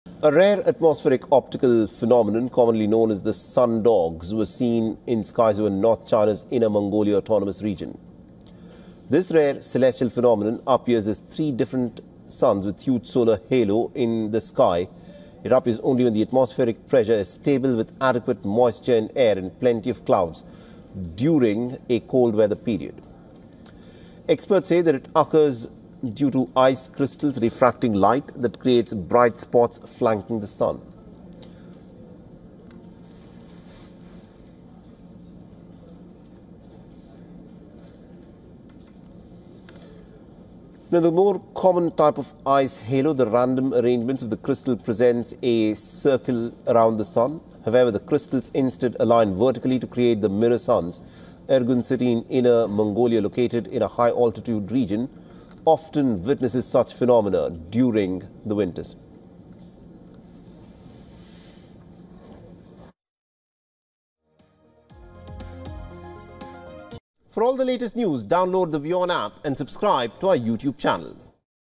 A Rare Atmospheric Optical Phenomenon Known as ‘Sun Dogs’ Seen in Skies Over Mongolia | WION
A rare atmospheric optical phenomenon known as ‘Sun Dogs’ has been observed in the skies over Mongolia, creating striking halos around the Sun. This video explains how Sun Dogs form due to ice crystals in the upper atmosphere, the science behind this stunning spectacle, and why such occurrences are uncommon. We also capture the breathtaking visuals and explore historical and cultural interpretations of Sun Dogs around the world. Whether you are a science enthusiast, photographer, or simply love natural wonders, this rare celestial display offers a unique opportunity to witness one of nature’s most fascinating atmospheric events.